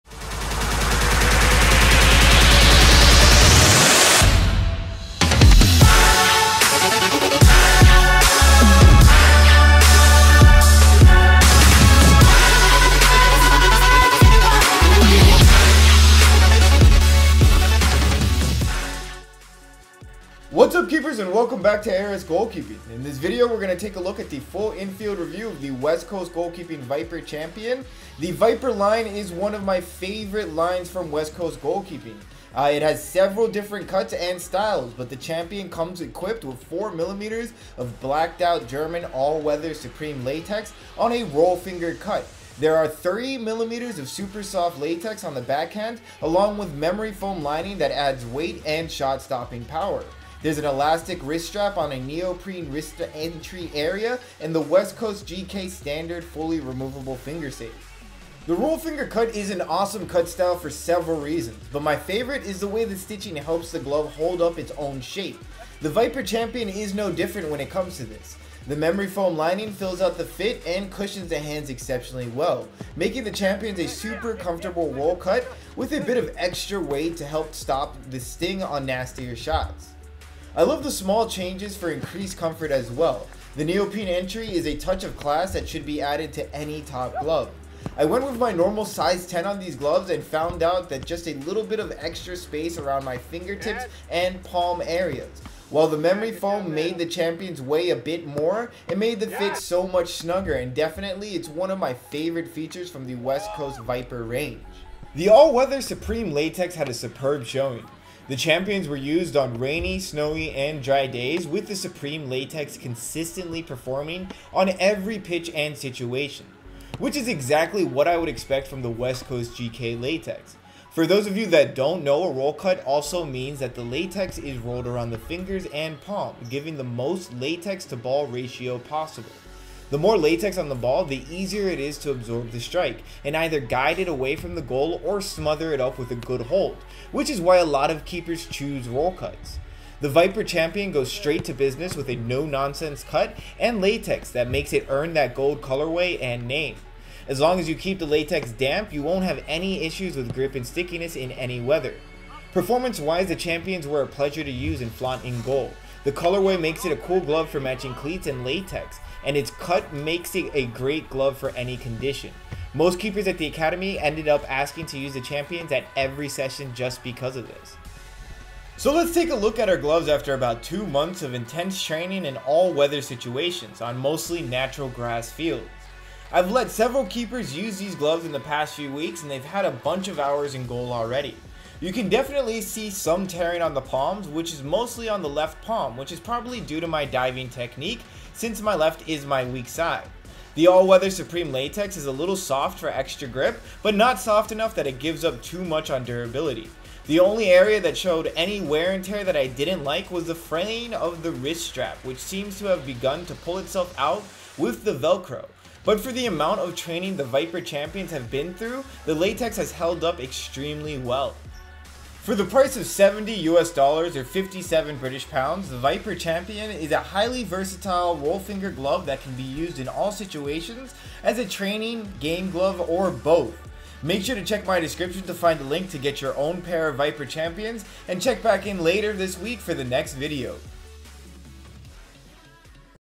West Coast GK Vyper Champion Goalkeeper Glove Review
The newest addition to the Vyper line from West Coast GK is the gorgeous gold Vyper champion which comes equipped with  4MM of all weather German Supreme latex on a roll finger cut. The backhand has 3MM of super soft latex coupled with a memory foam lining for a super comfortable and snug fit.

MUSIC